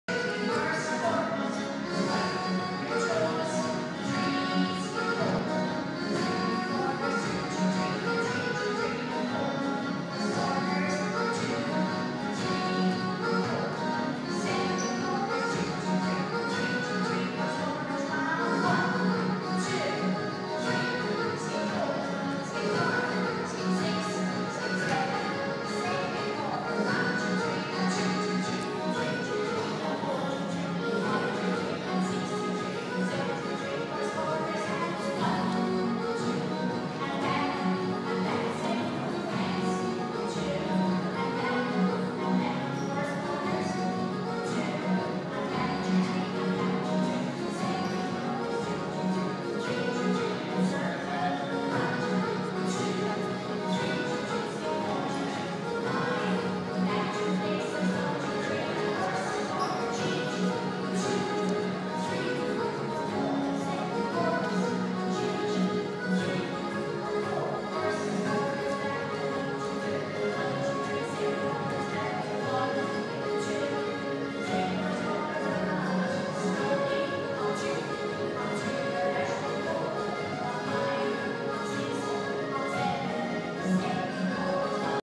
Aisling at Dancing Display 2012
Aisling is at the back of the hall with her green headband on lol and near the basketball hoop.